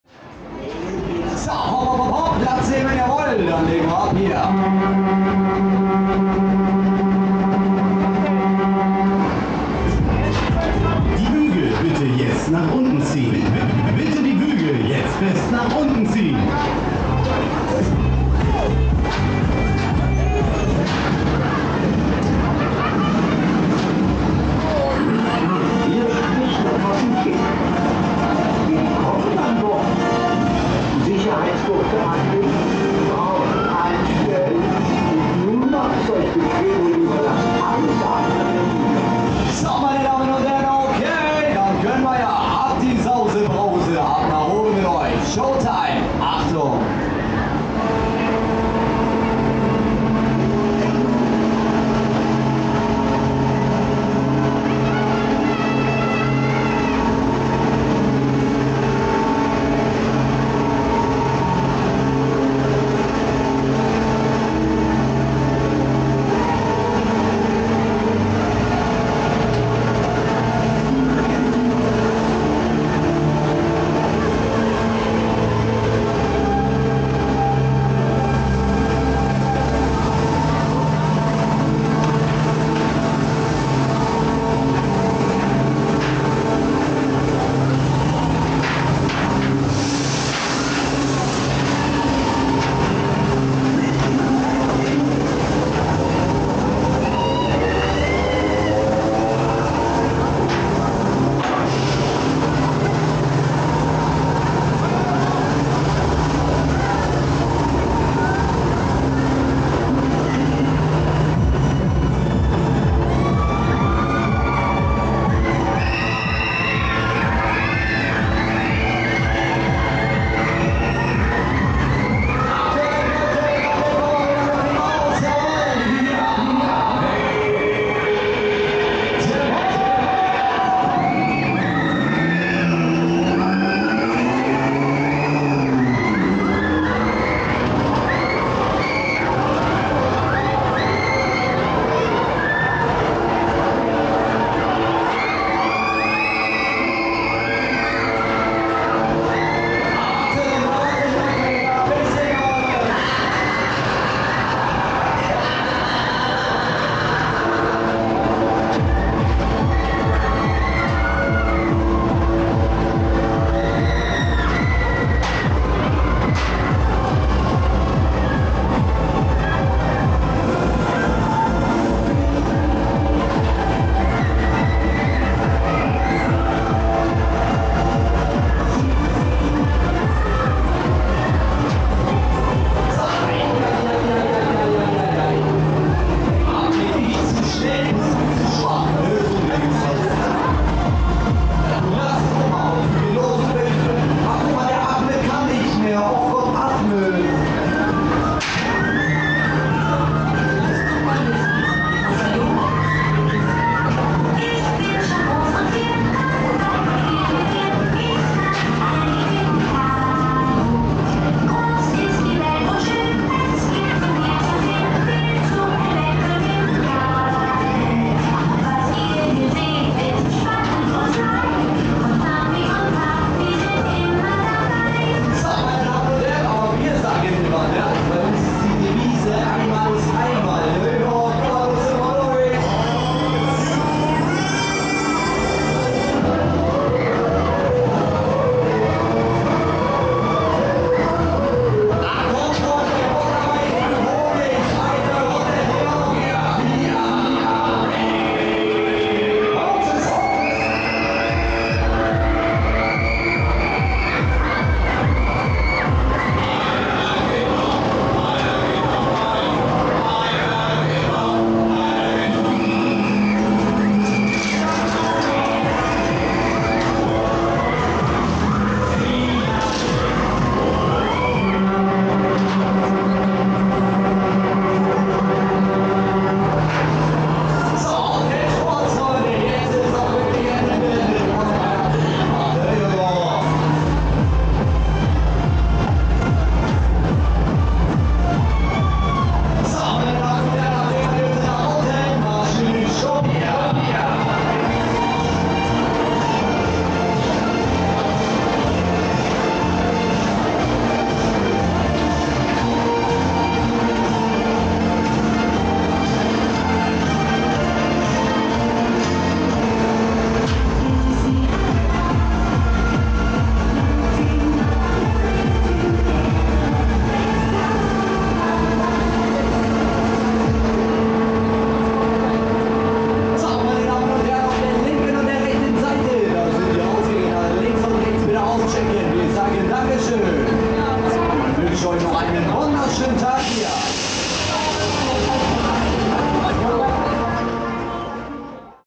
Predator - Kaiser - Offride - 4K - Augsburger Osterplärrer 2024
This ride is seriously intense! Sustained strong negative g's are something else, you have to be alright with the feeling of blood rushing to your head. It's not the most intense ride in the world don't get me wrong, but it still is up there. Not gonna lie. Sorry KMG, but the Move-It models can not provide this ride experience.

This is Predator by german showman Kaiser. Soriani & Moser built this ride, calling this ride model the "Top Star Tour".

Incredible machine with incredible forces! Awesome hangtime with strong negative g's. You need strong thighs since the restrains are pretty hard, and you get pressed into them. I don't mind them at all, I find it pretty comfortable since I have some meat there. If the restrains are hard like that I think it elevates the feeling of hangtime.

The design is awesome, too. While I'm not a fan of using an IP to name the ride, the color scheme is beautiful! The lights give it an awesome atmoshpere, too!
The ride operator is on this ride for his second year I think (at the time of uploading) and he still got it. Great show, great cycle. Way better then before, I had quite some complaints. Not anymore, this is brilliant!